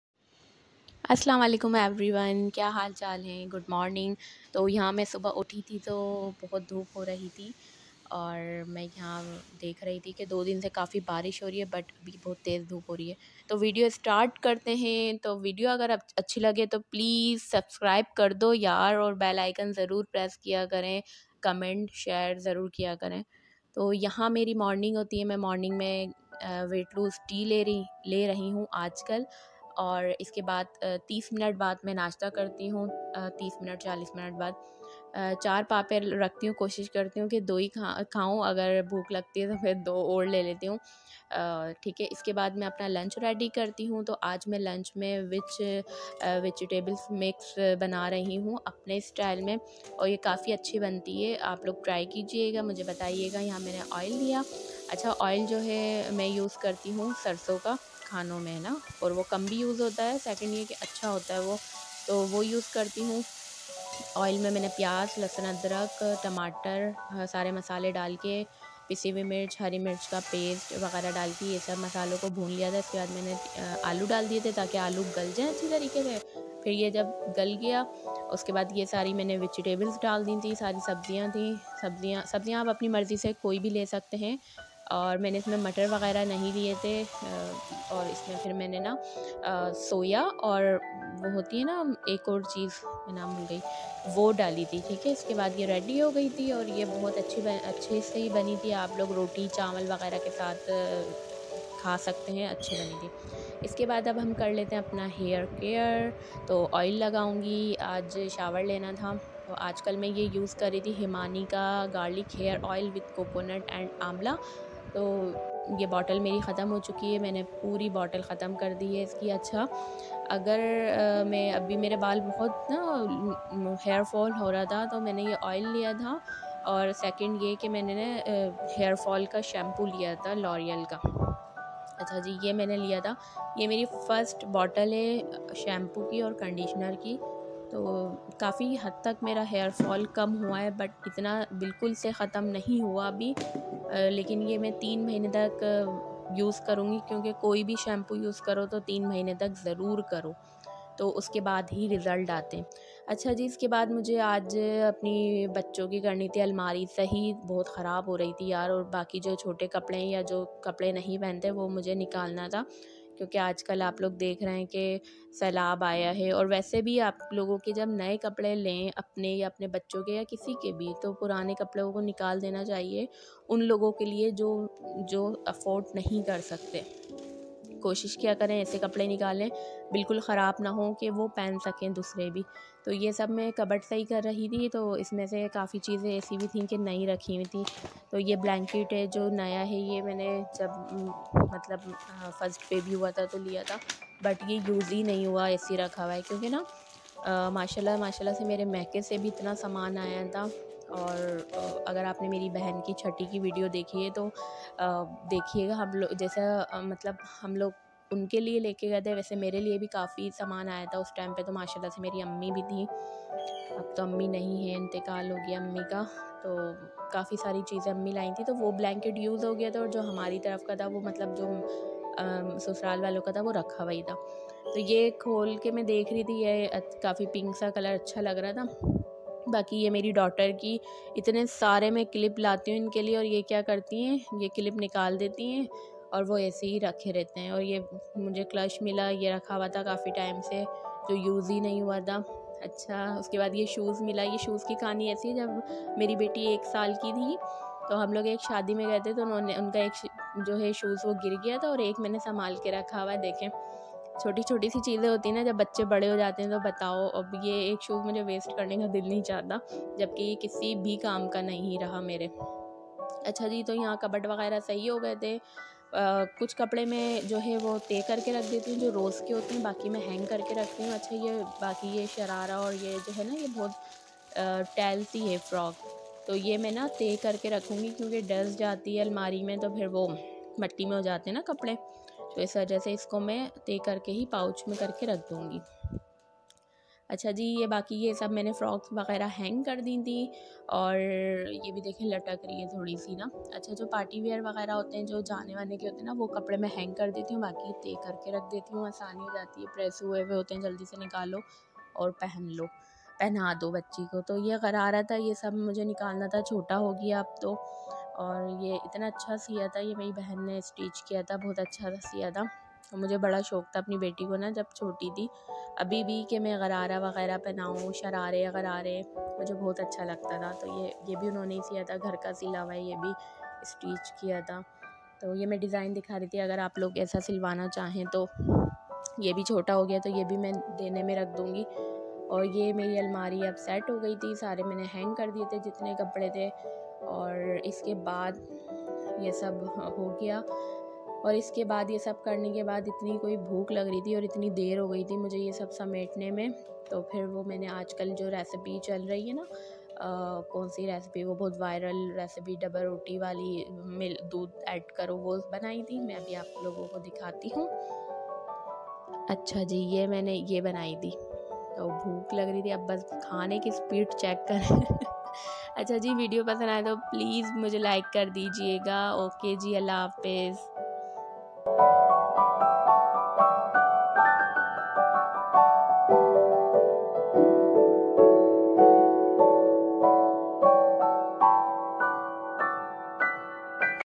Today Vlog | Hemani Hair Oil & Loreal's Shampoo Review | Aj Hoi Bati Ki Almari Ki Safi | Mix Sabzi
Hellooo . . . ! ! !
Ajj Ka Vlog Daily routine say related hai jis main hum ny hemaniherbal hairoil Ka Review Kara Aur loreal shampoo Ka Review kara Aur mixsabzi Banai Specially bati ki almari set ki
umeed hai apko ajj ka vlog pasnd aye ga
jazakALLAH
DUA main Yaad Rakhiye Ga ALLAH NIGHAYBAN